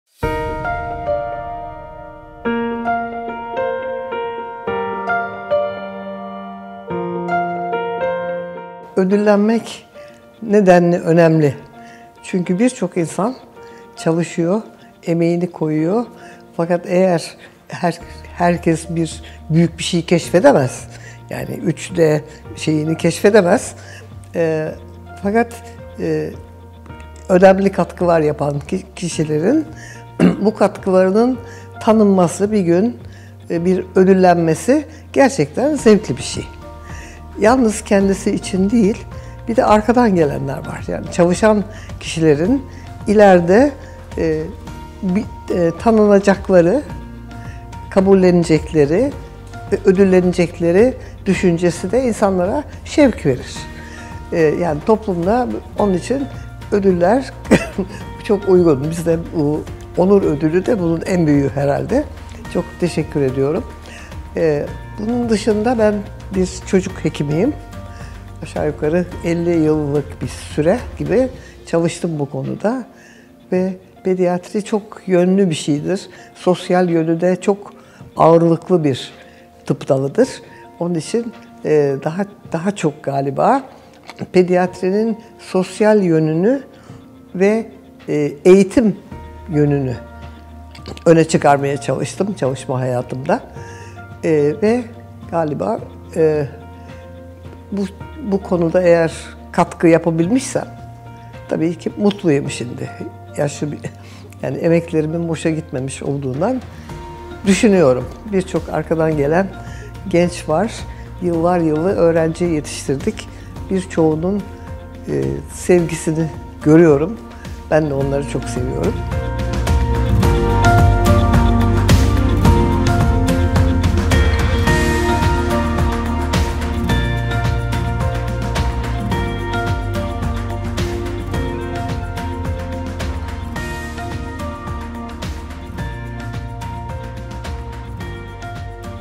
2017 Dr. Nejat F. Eczacıbaşı Tıp Onur Ödülü | Prof. Dr. Olcay Neyzi
2017 Dr. Nejat F. Eczacıbaşı Tıp Onur Ödülü’nü kazanan Prof. Dr. Olcay Neyzi tıp alanında ödüllendirilmenin önemini anlatıyor. Ödül kazanan tüm bilim insanlarını kutluyoruz.